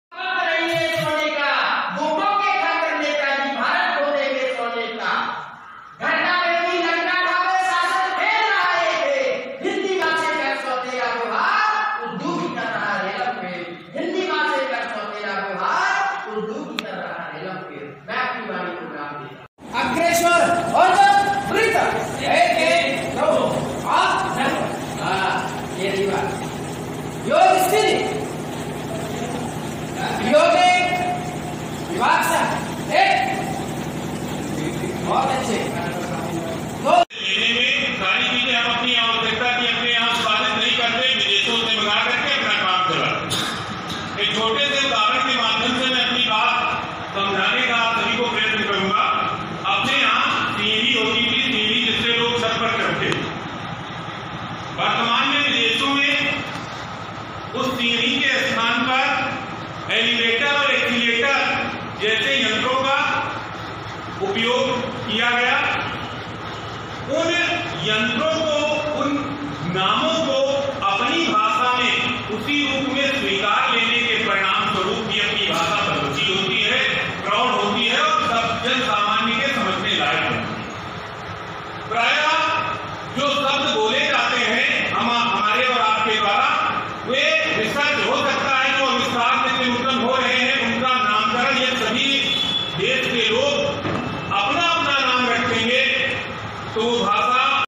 बाराबंकी : स.शि.म. तहसील फतेहपुर में धूमधाम से मनाया गया हिंदी दिवस
बाराबंकी : सरस्वती शिशु मंदिर तहसील फतेहपुर में धूमधाम से मनाया गया हिंदी दिवस